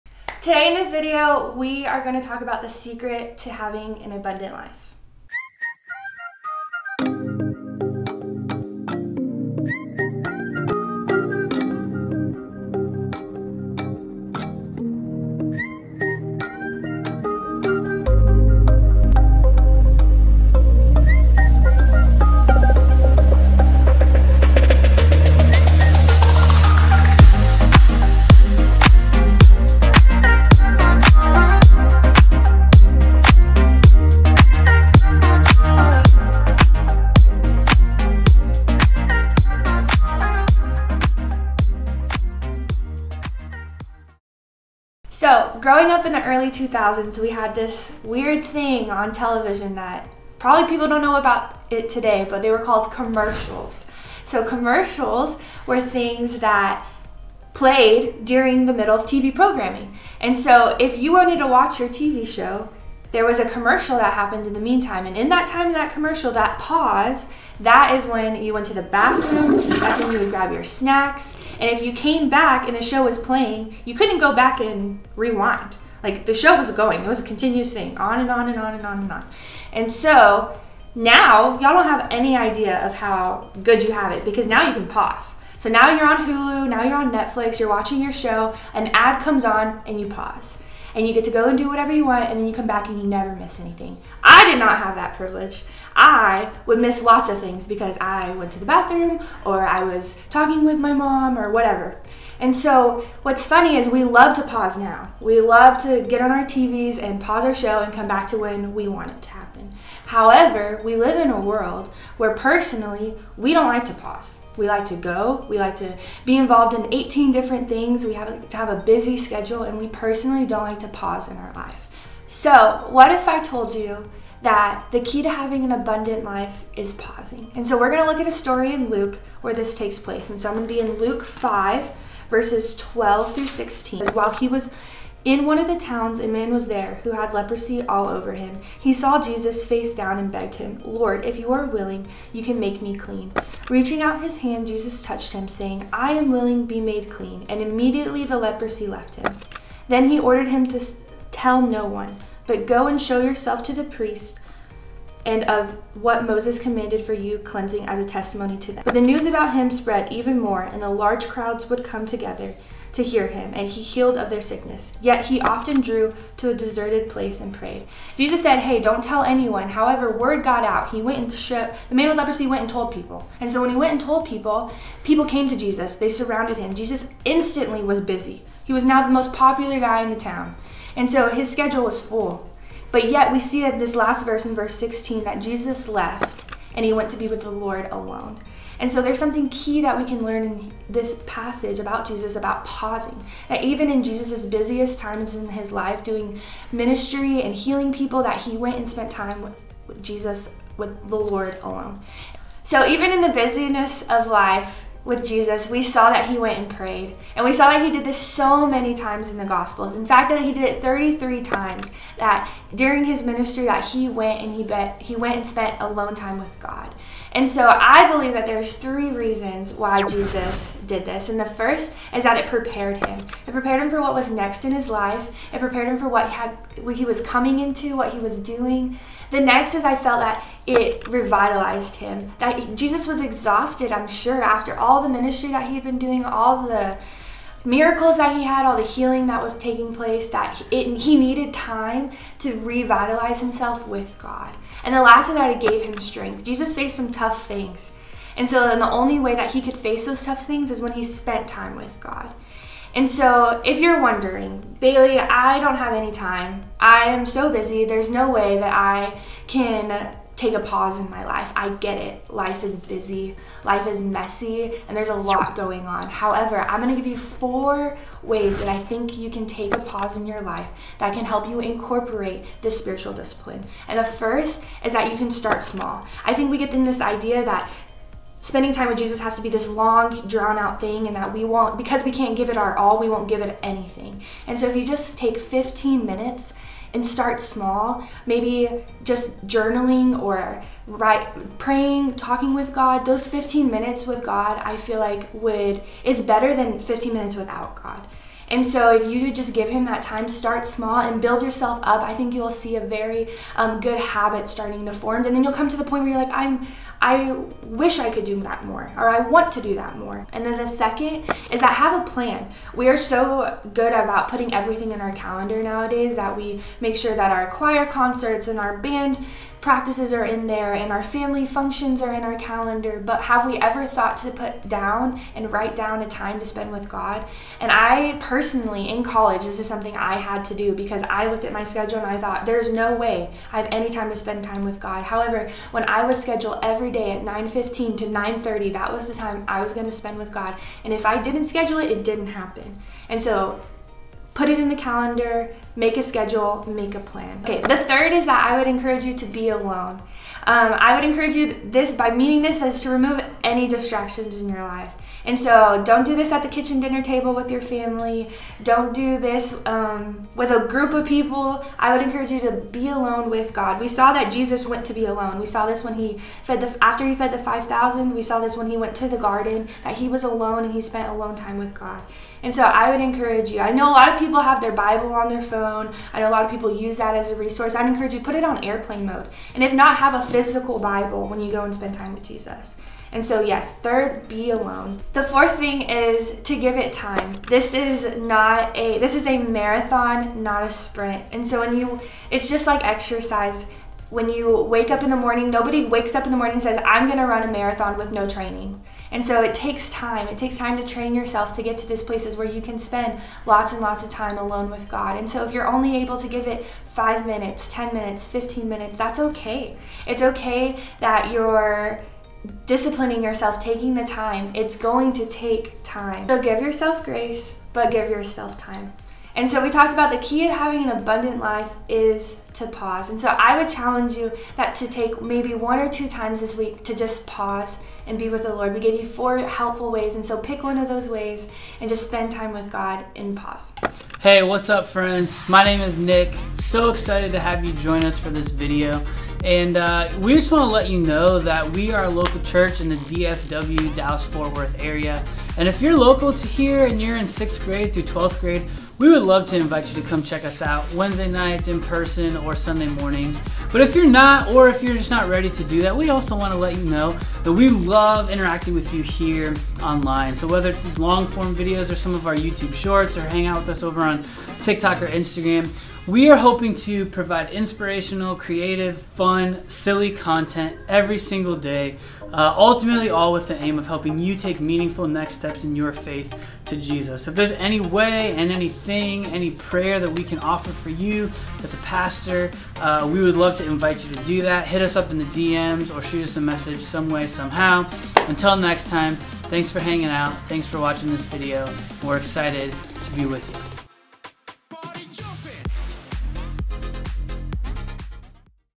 4 Hacks to Slowing Down & Learning to Pause
You might be surprised, that to live out the "Jesus Life" in a FULL and ABUNDANT way, actually requires you to do the opposite, and slow down.

It requires the use of withdrawing, pausing and getting away from distractions and obstacles.

You might be surprised to learn that Jesus withdrew from the crowds and went away to pray alone a recorded 33 times throughout the Bible.

Shocking?!

So what can we learn from him?
And furthermore, how can we incorporate pausing more effectively in our lives?

00:00-00:45 Intro
00:45-01:56 Pausing on TV used to not be possible
01:56-03:05 The Key to an Abundant Life is Pausing
03:05-04:29 The Surprising Response of Jesus to Busy Demands on his time
04:29-05:15 HACK 1: Start Small
05:15-05:52 HACK 2: Have a Plan
05:52-06:32 HACK 3: Do it Alone
06:32-07:19 HACK 4: Give it Time
07:19-08:25 Outro